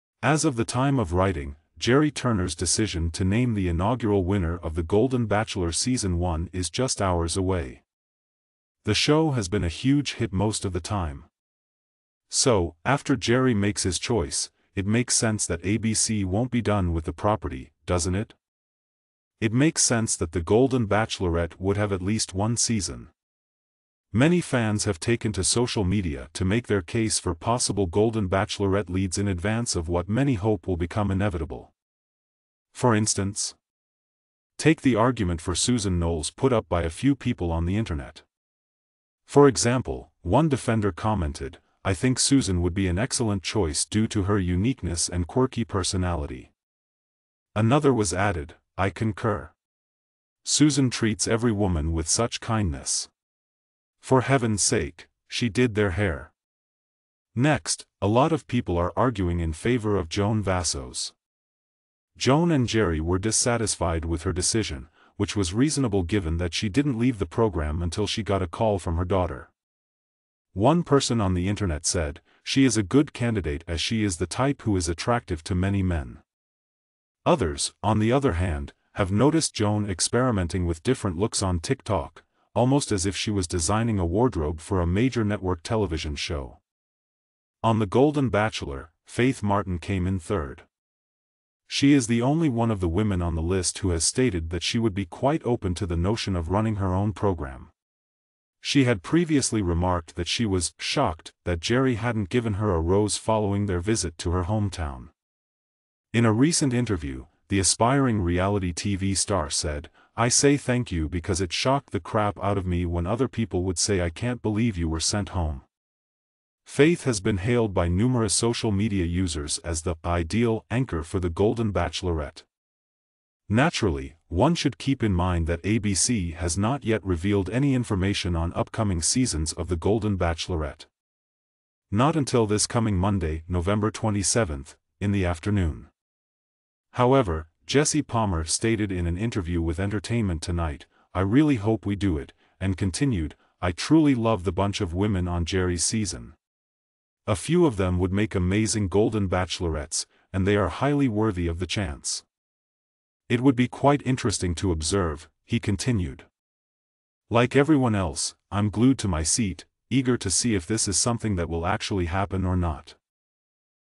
The Golden Bachelorette Season 1 Winner Revealed! Fans Make Their Picks!
As of the time of writing, Gerry Turner's decision to name the inaugural winner of The Golden Bachelor season one is just hours away.
The show has been a huge hit most of the time.
So, after Gerry makes his choice, it makes sense that ABC won't be done with the property, doesn't it?
It makes sense that The Golden Bachelorette would have at least one season.
Many fans have taken to social media to make their case for possible Golden Bachelorette leads in advance of what many hope will become inevitable.
For instance?
Take the argument for Susan Noles put up by a few people on the Internet.
For example, one defender commented, "I think Susan would be an excellent choice due to her uniqueness and quirky personality."
Another was added: "I concur! Susan treats every woman with such kindness! For heavens sake, she did their hair!
Next, a lot of people are arguing in favor of Joan Vassos.
Joan and Gerry were dissatisfied with her decision, which was reasonable given that she didn't leave the program until she got a call from her daughter.
One person on the internet said, "She is a good candidate as she is the type who is attractive to many men."
Others, on the other hand, have noticed Joan experimenting with different looks on TikTok—almost as if she was designing a wardrobe for a major network television show.
On The Golden Bachelor, Faith Martin came in third.
She is the only one of the women on the list who has stated that she would be quite open to the notion of running her own program. She had previously remarked that she was "shocked" that Gerry hadn't given her a rose following their visit to her hometown.